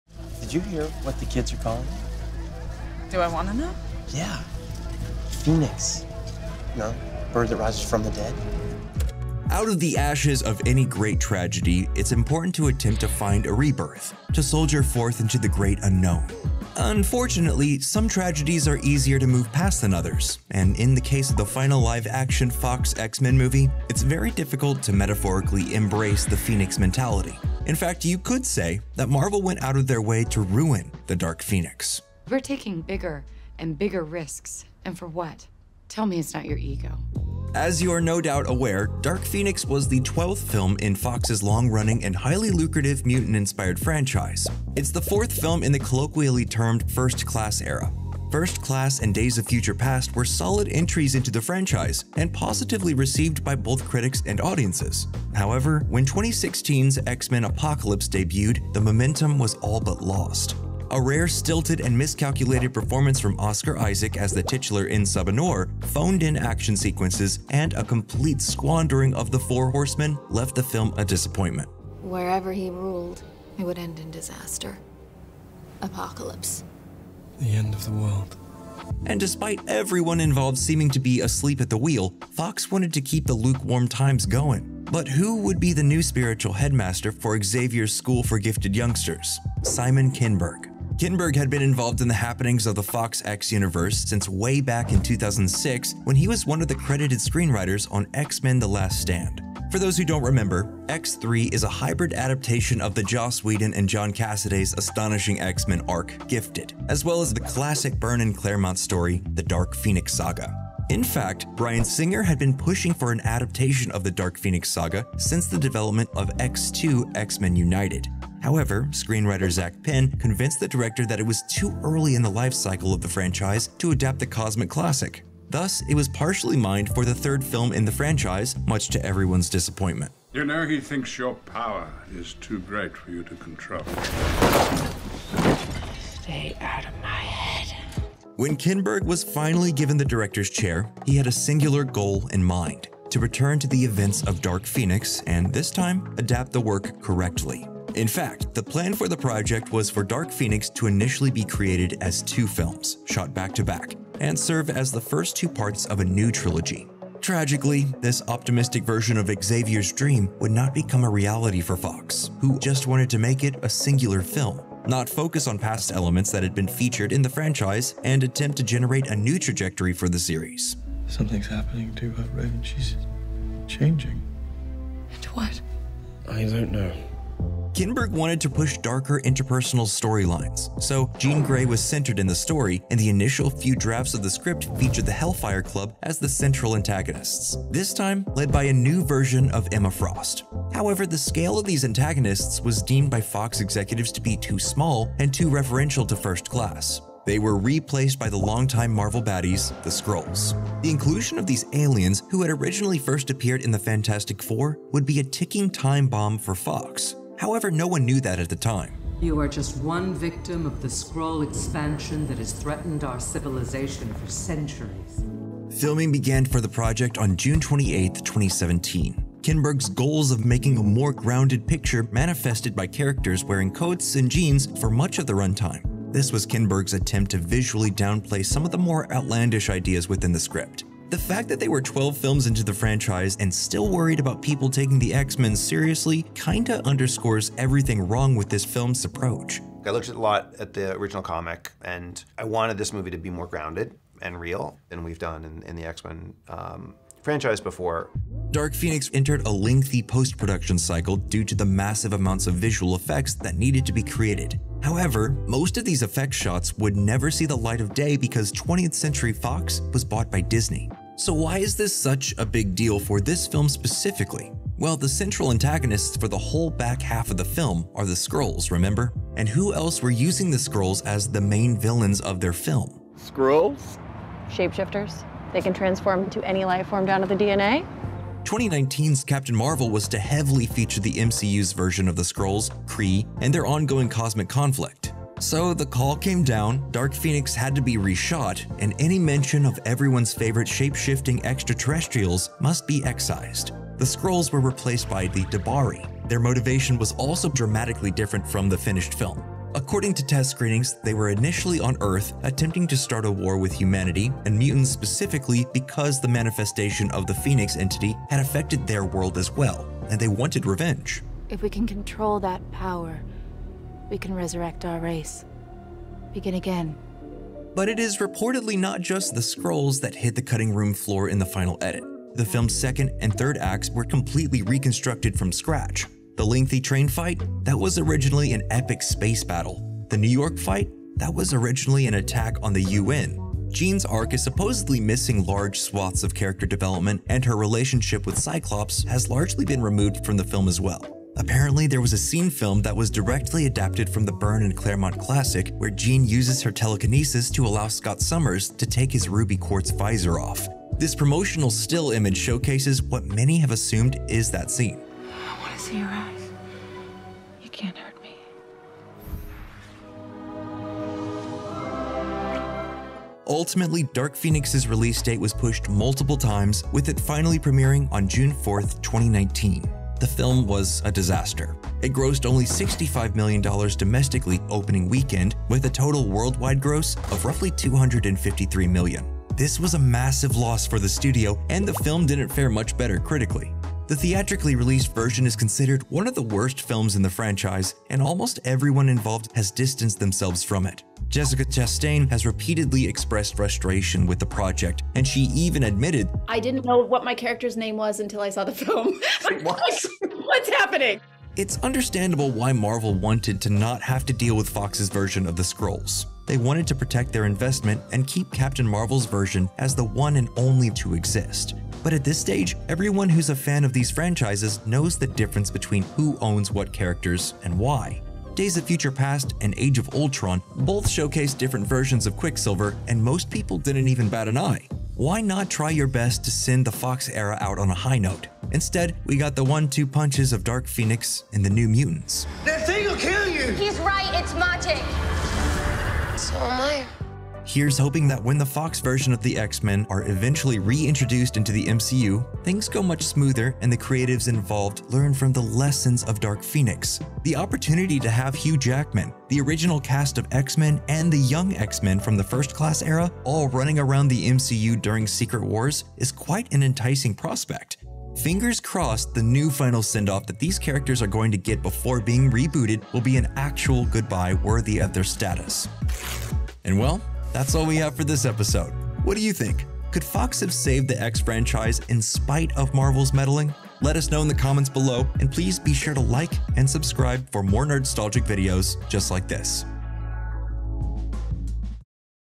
How THIS Became The Worst X-Men Movie
X-Men Dark Phoenix was the 12th film in Fox’s long-running mutant-inspired franchise, and it’s the fourth film in the colloquially termed “First Class” era.  Though after the success of almost a dozen X-Men films the franchise was starting to lose steam fast.  But how exactly did the X-Men franchise fall from grace so hard?  Was X-Men Dark Phoenix set up to fail?